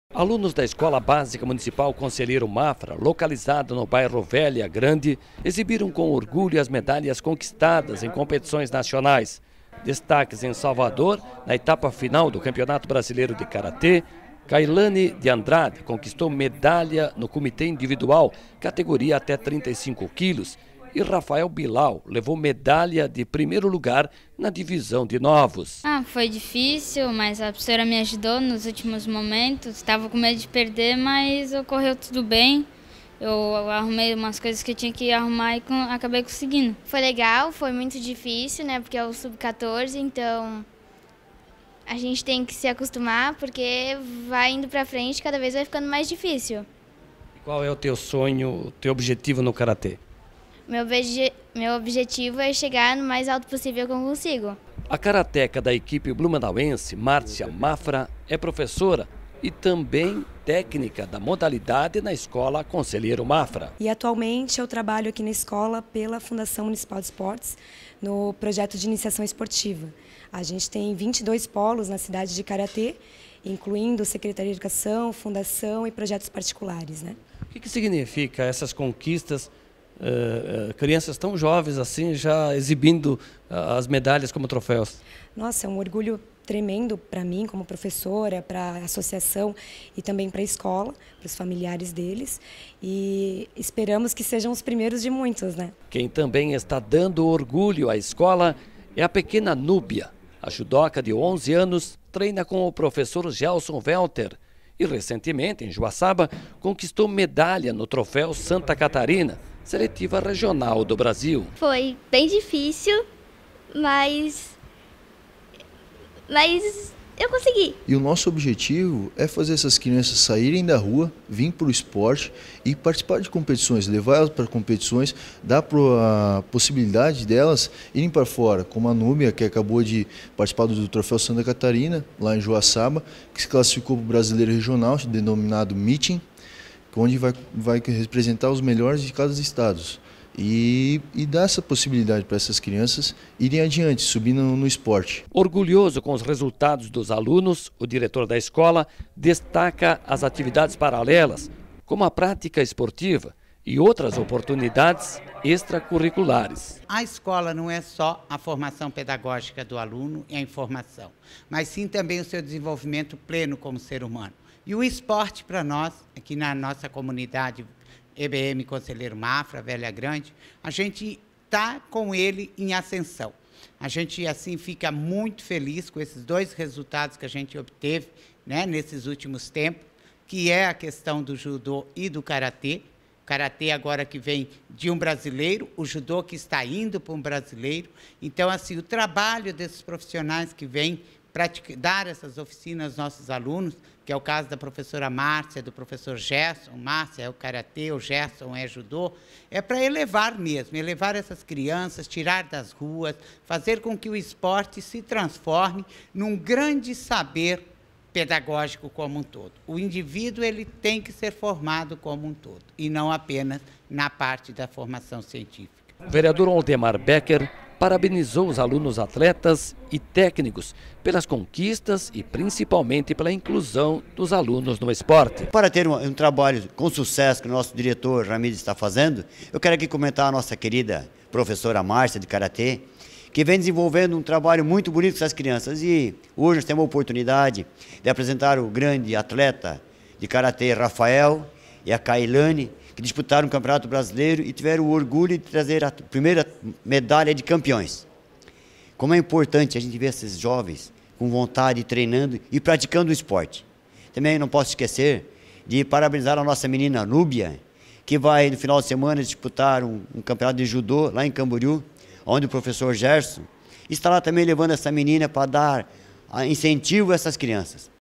Alunos Medalhistas
Alunos da Escola Básica Municipal Conselheiro Mafra, ganham medalhas no Karatê e no Judô.